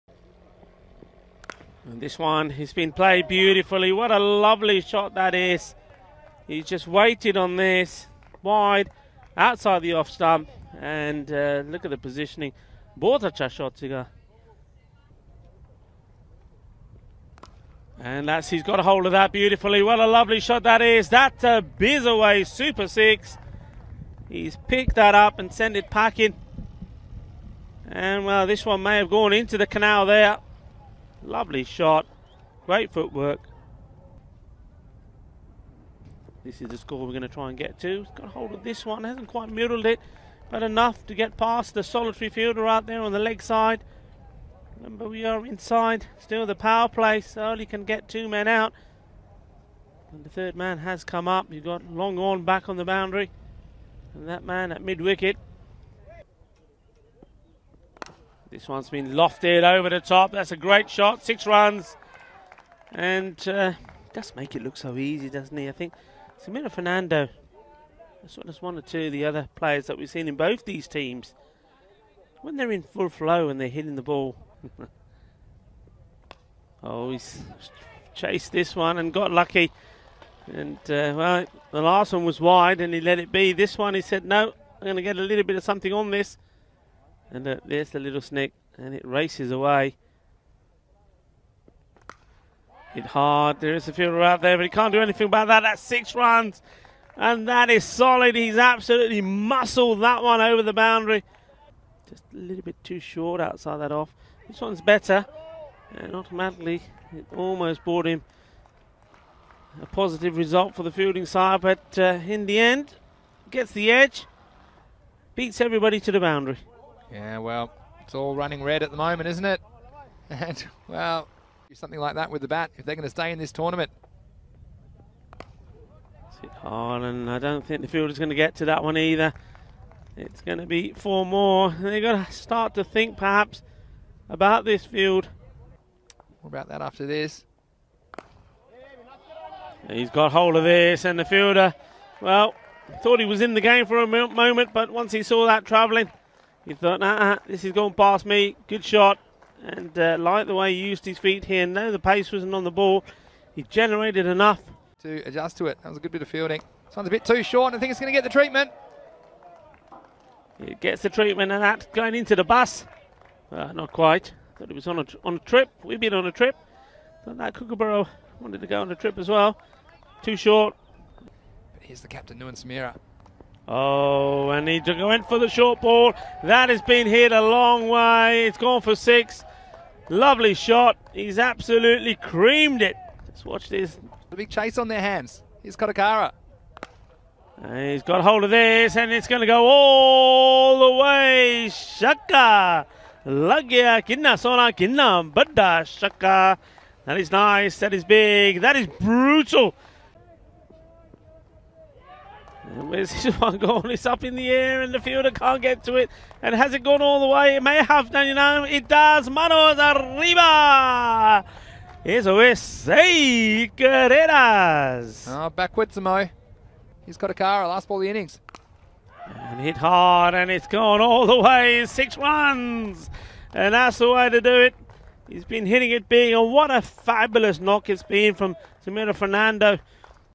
Some performance from Sameera Fernando for Royal Cricket Padova
Man of the Match: Sameera Fernando 86*(34) & (1/10) - PAD vs RCP | Highlights | Fancode ECS Italy Day 5 | Venice 2021 | ECS21.191